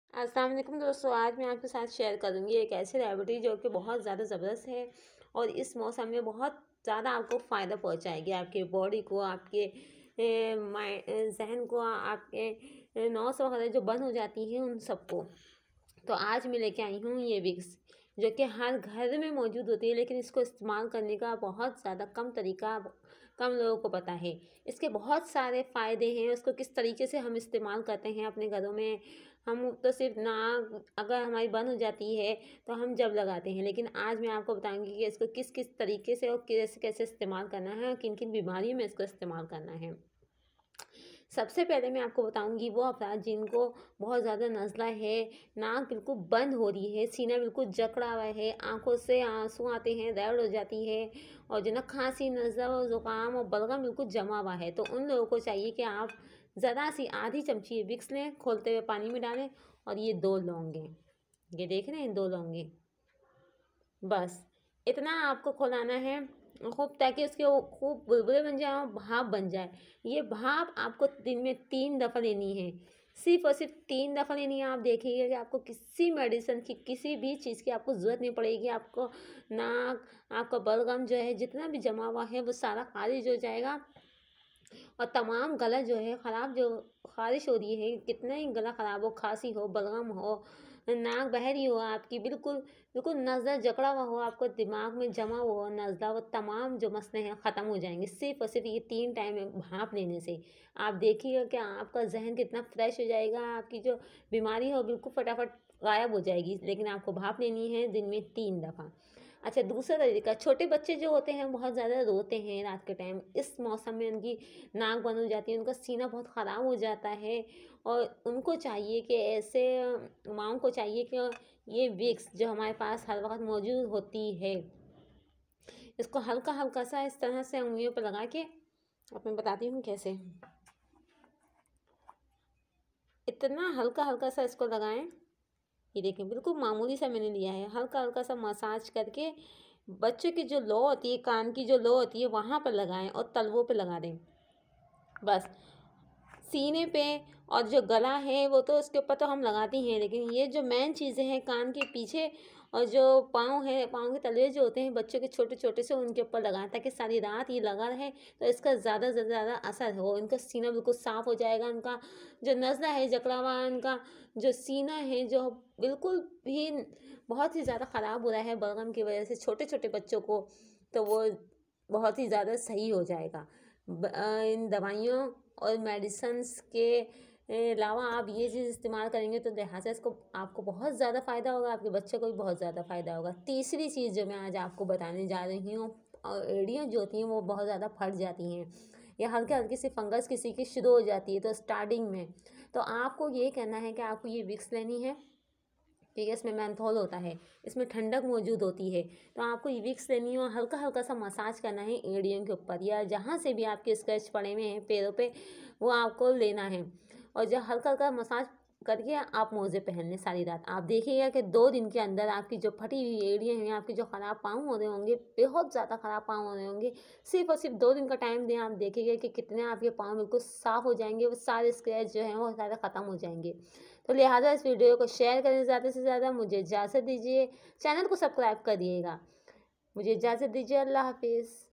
Band nak khole Ser derd door karen or gale ki her taklef door karen sirf 2 dafa steem le
Band nak khole Ser derd door karen or gale ki her taklef door karen sirf 2 dafa steem le#Ghar main pari weeks by